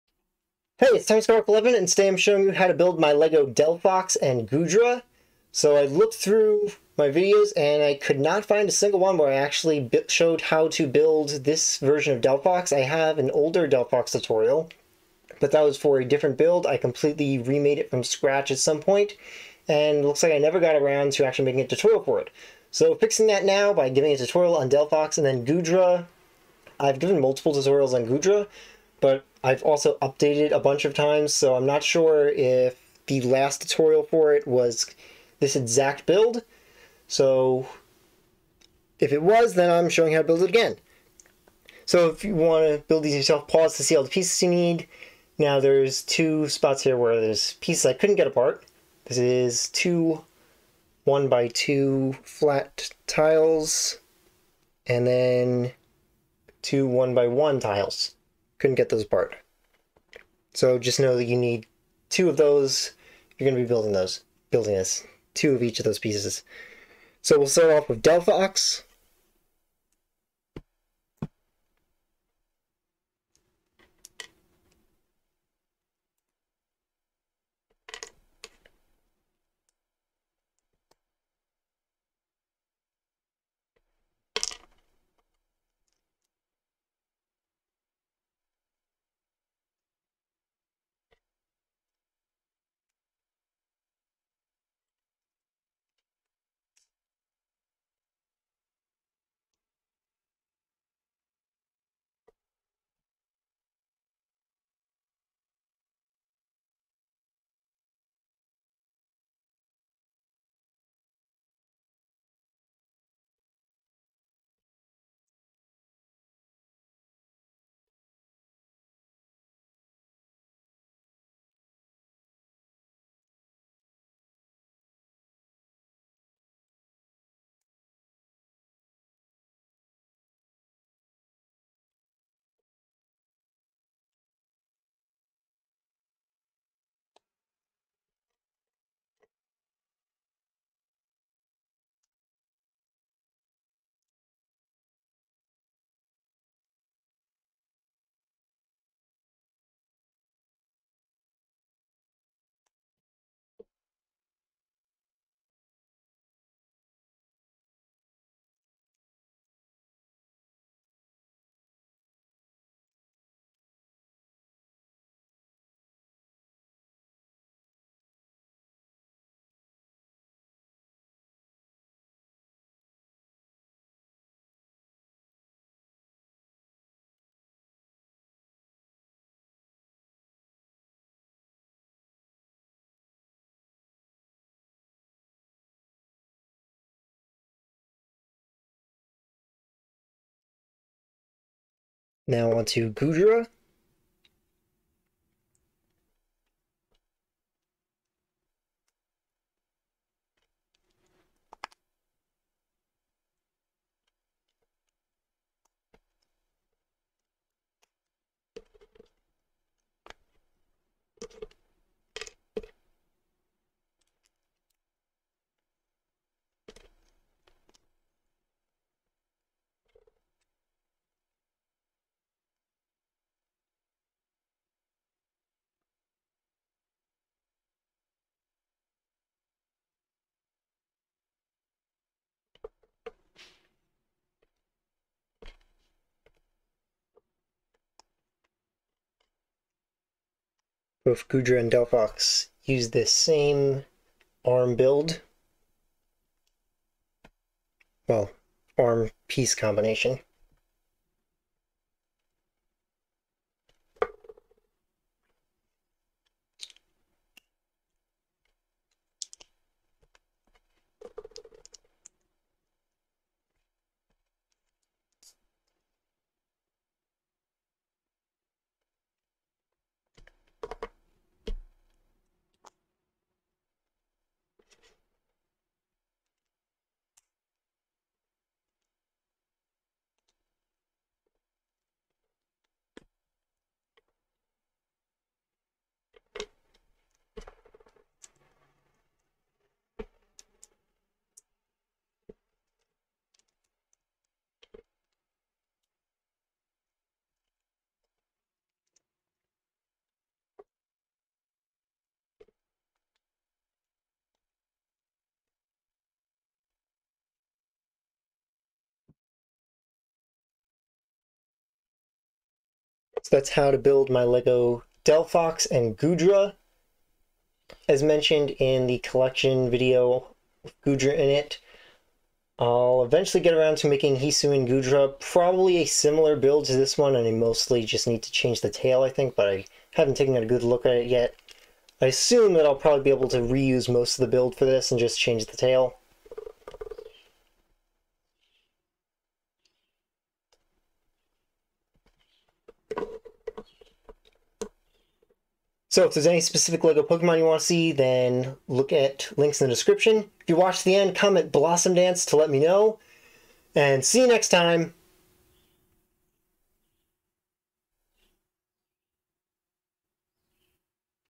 How to Make LEGO Pokemon: Delphox and Goodra
For LEGO previews and commissions

0:00 Intro and parts
1:00 Delphox
3:10 Goodra